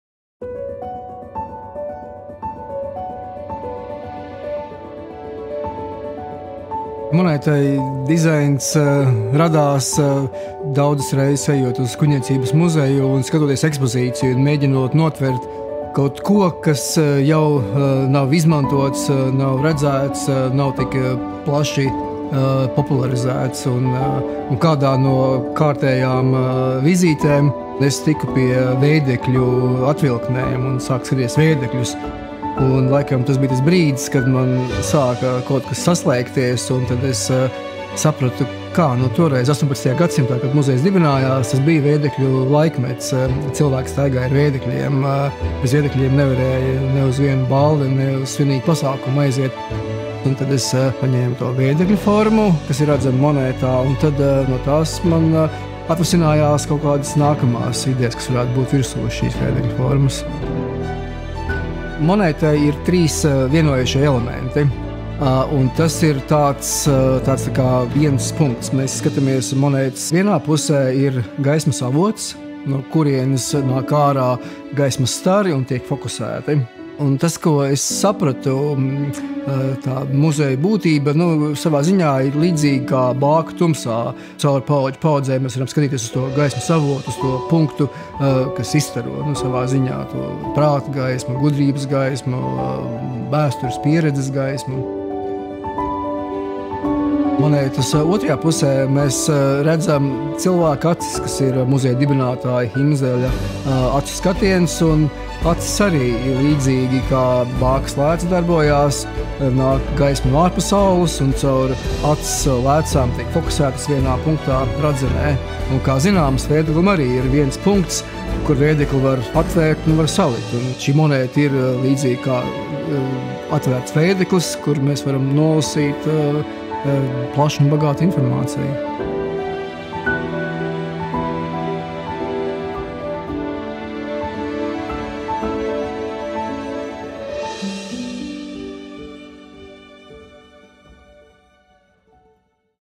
Kolekcijas monēta "Gaismas staru vēdeklis"
Latvijas Banka 29. novembrī par godu Rīgas vēstures un kuģniecības muzeja 250. gadadienai izlaiž neparastas formas sudraba kolekcijas monētu "Gaismas staru vēdeklis".

Monētas grafiskā dizaina autors ir mākslinieks Ivars Drulle.

Iedvesmojoties no vēdekļiem, kas glabājas muzeja krājumā un bija raksturīga sieviešu garderobes sastāvdaļa laikmetā, kad tika izveidota muzeja krājuma pamatā esošā Rīgas ārsta Nikolausa fon Himzela kolekcija  un dibināts muzejs, mākslinieks izvēlējies monētai neparastu – vēdekļa – formu.

Monētas aversā atveidots gaismas avots, no kura vēdekļa veidā izplatītos gaismas starus savāc lēca, tos pastiprinot un fokusējot vienā virzienā. Lēca simbolizē muzeju – zināšanu krātuvi, kas izplata gaismu: paplašina redzesloku un palīdz izzināt.

Monētas plastiskā veidojuma autore ir Ligita Franckeviča.

Jauno monētu varēs iegādāties Latvijas Bankas numismātikas produktu iegādes vietnē Monētas cena – 72.00 eiro, iegādes limits vienai personai – 5 monētas.

Monētas tirāža ir 3000 eks. Monēta kalta Nīderlandes kaltuvē Koninklijke Nederlandse Munt.